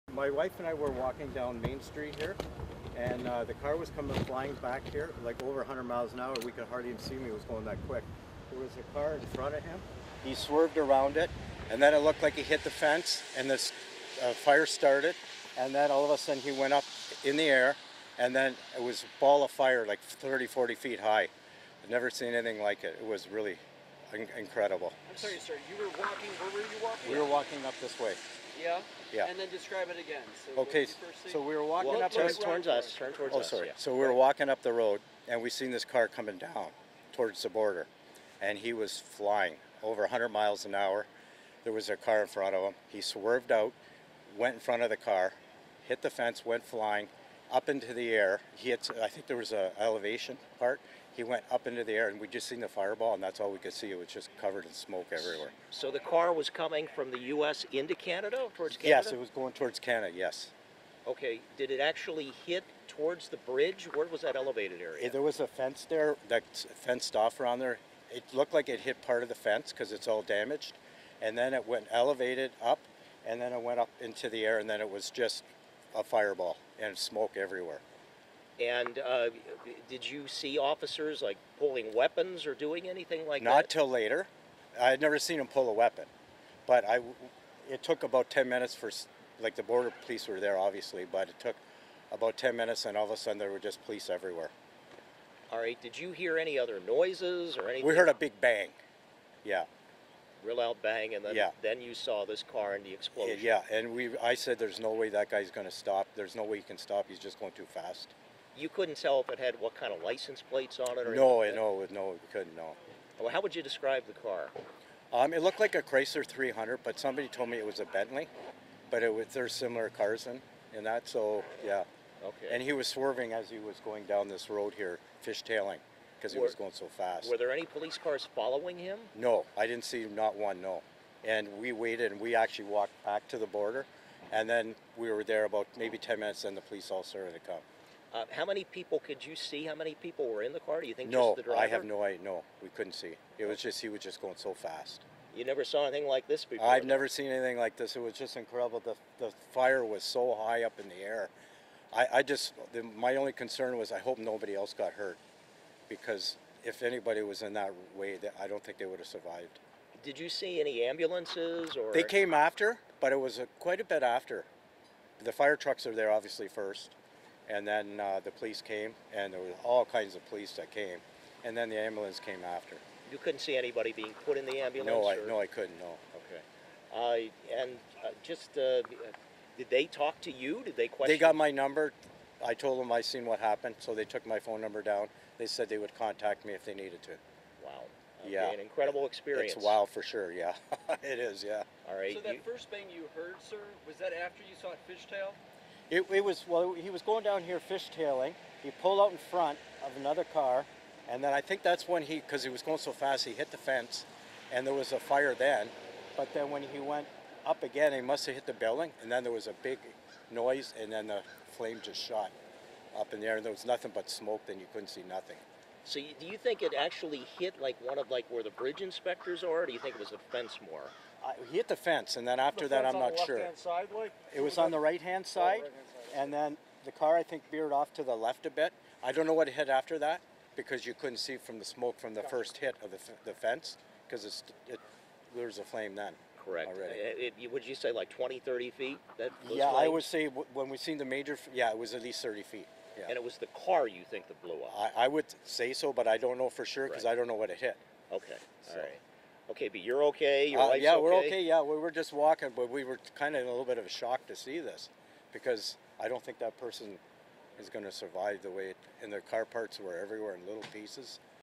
Witness describes Niagara Falls explosion at the Rainbow Bridge
Witness describes Niagara Falls explosion at the Rainbow Bridge: Mike Guenther from Ontario was walking with his wife near the Rainbow Bridge in Niagara Falls when he says they saw a car driving at a high rate of speed before crashing.